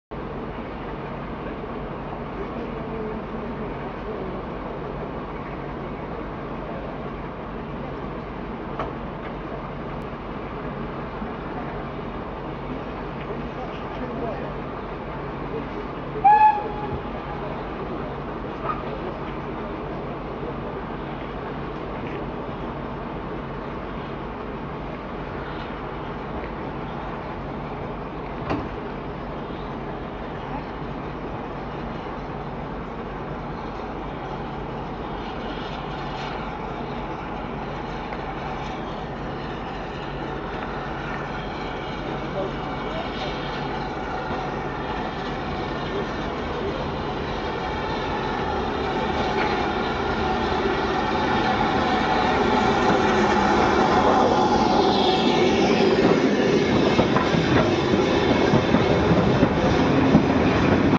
80078 arrives at Harmans Cross
80078 arrives at Harmans Cross on the 13th May 2007.